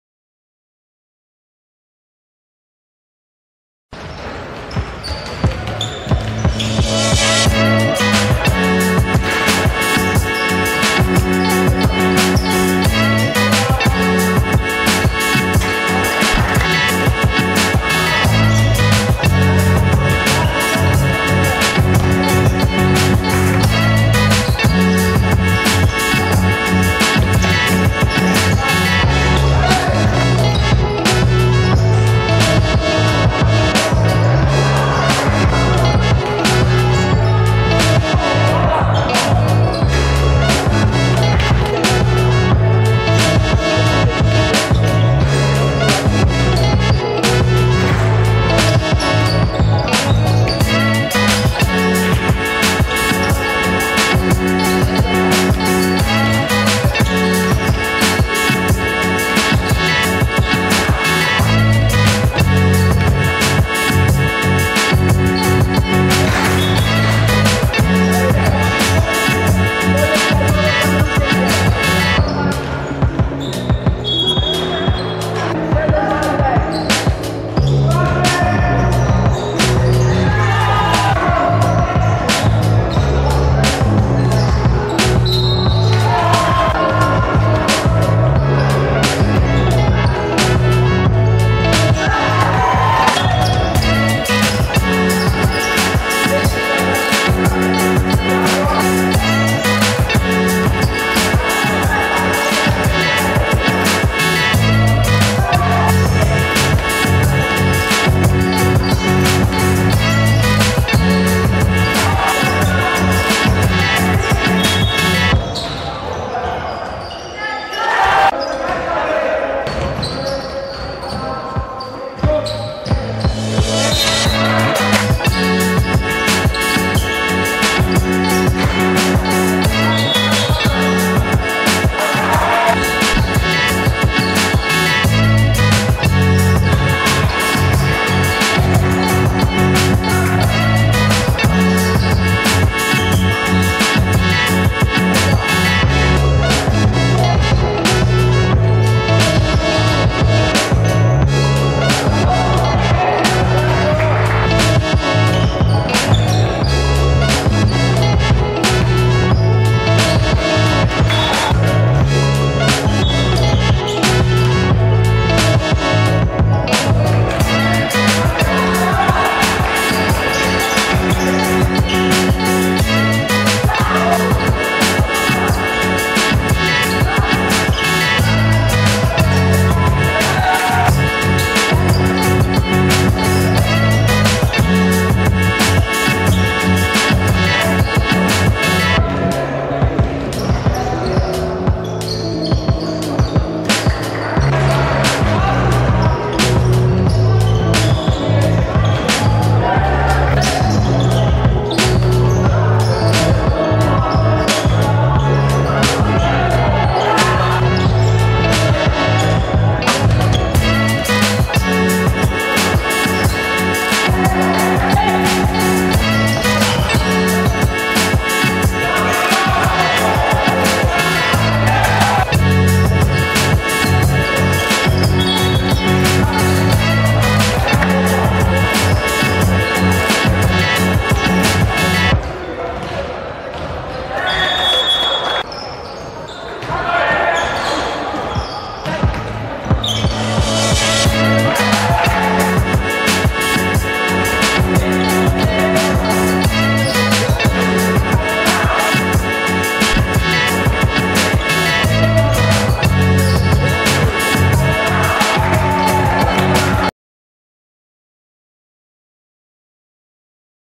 04 03 Knights vs Cougars – Rookie Div Zima 2022 23
Knights 57:43 Cougars
I.Stelmach 14pkt 7zb 11ast 4stl
M.Jordan 20pkt 8zb